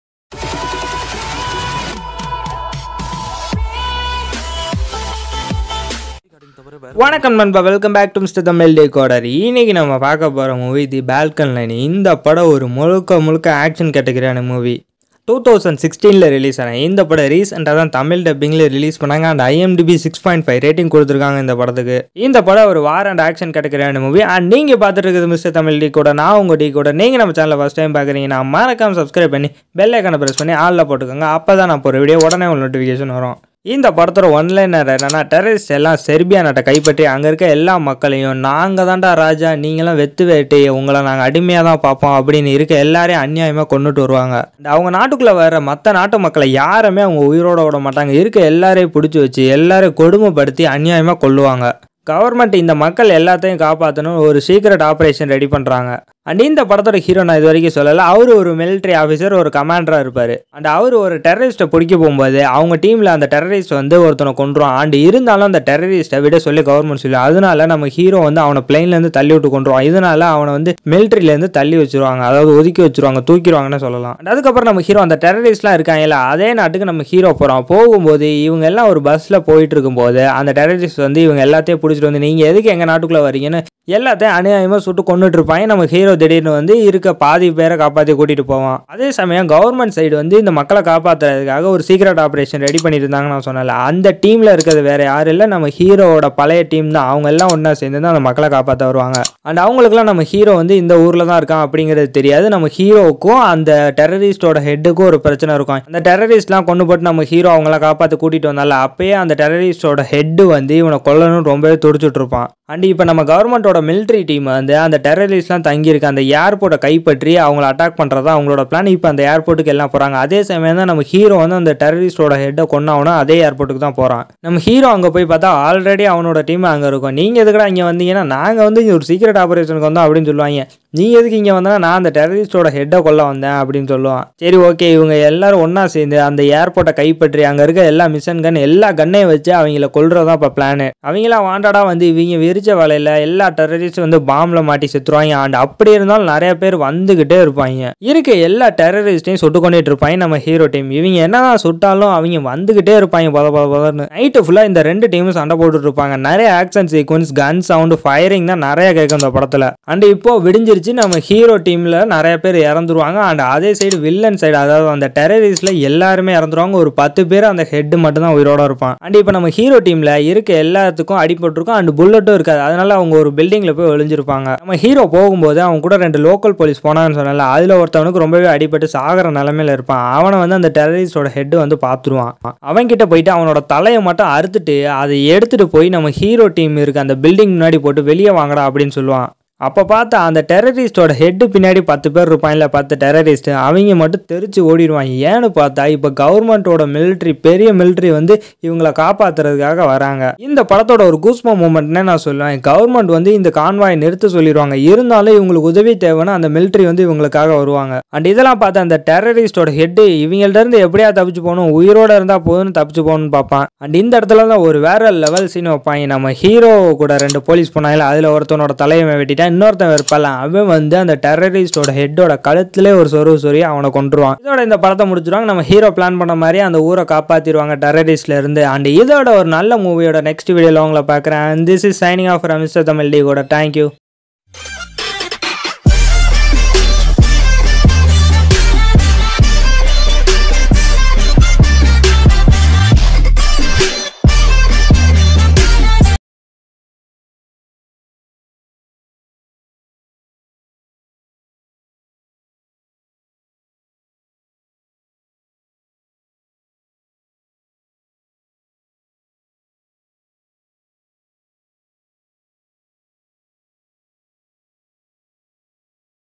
The Balkan Line தமிழ் Dubbed Hollywood Movie Explained/Review by Mr Tamildecoder Best Action Movie
Tamildubbed Hollywood movie review
Tamil movie review
Hollywood movie review and Explains
Hollywood movie tamil
Hollywood movie Explained by tamil